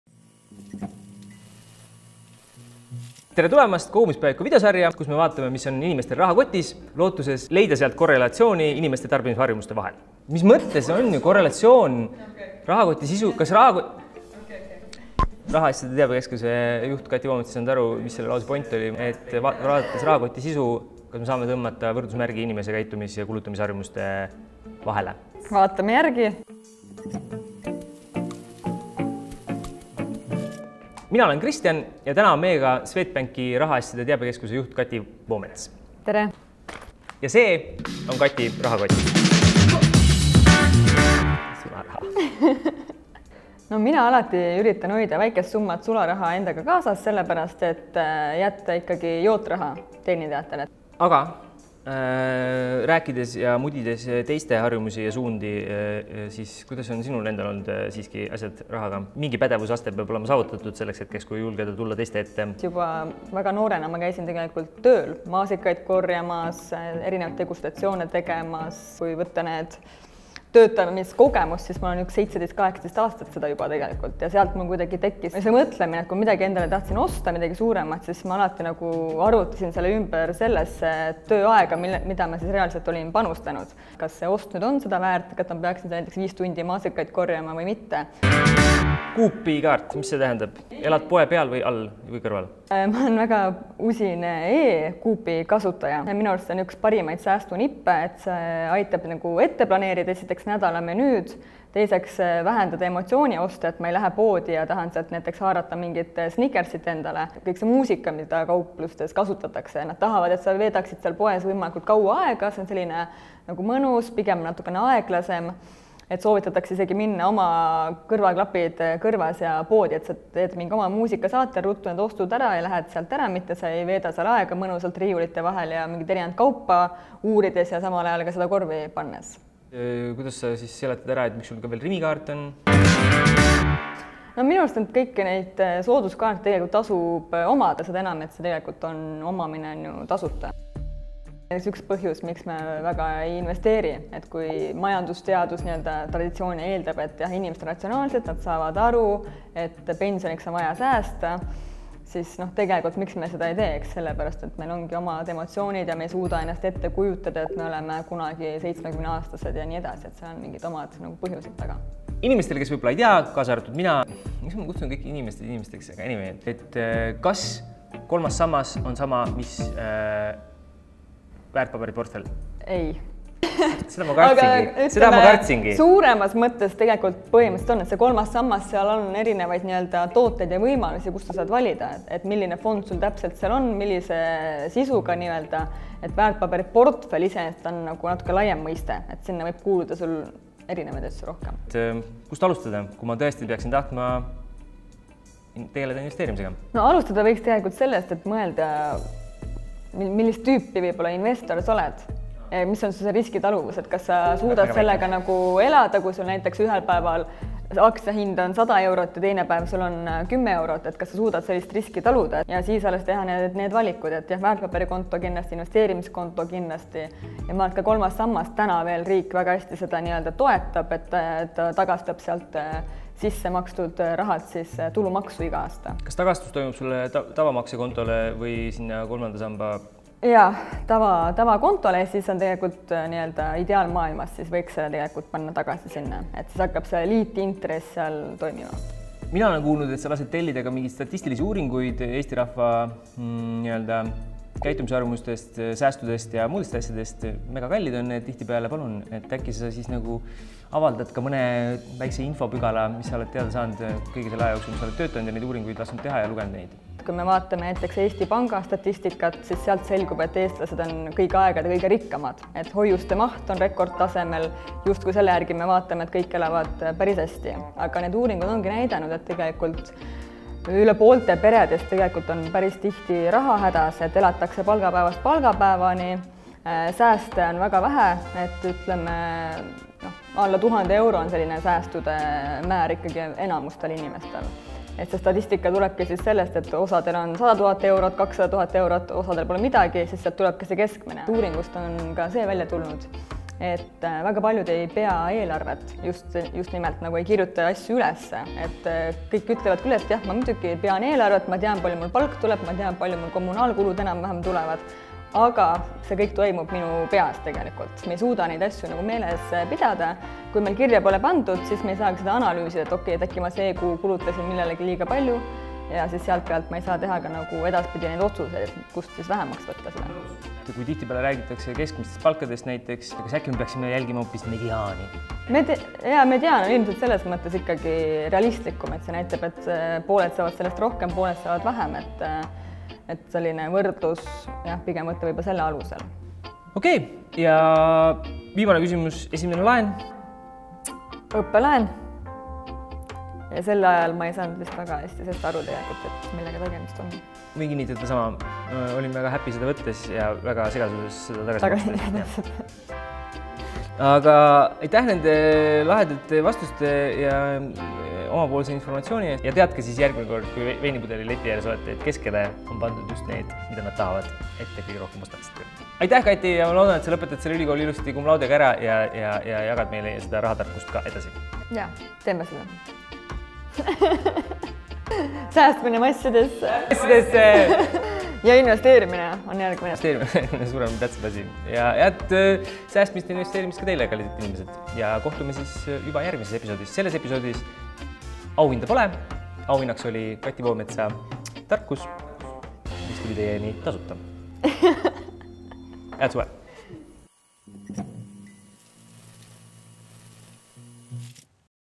Kogumispäevik: rahatarkuse eri! Kui suured on eestlaste säästud tegelikult?
Võida endale uhiuus Craftory rahakott! Pane kommentaaridesse kirja mõni nipp, kuidas Sina raha säästad ning võid juba 19.07 olla uhiuue rahakoti omanik!

Kuigi videos sai öeldud, et seekord auhinda pole, otsustasime auhinna siiski tublide kommenteerijate vahel välja loosida :)

Uues Kogumispäeviku episoodis uurime, mis on SwedbankiRahaasjade Teabekeskuse juhi Kati Voometsa rahakotis. Saame teada, miks tasuks poodi minna just oma kõrvaklappides mängiva muusika saatel ning mida peaks endale selgeks tegema enne investeerimisega alustamist.

Pane Sinagi kommentaaridesse mõni oma nipp, mis aitab Sul raha kokku hoida. Kui soovid kõrva taha panna veel hulgaliselt kavalusi, liitu ka populaarse Facebooki grupiga Kogumispäevik või jälgi Instagramis @kogumispaevik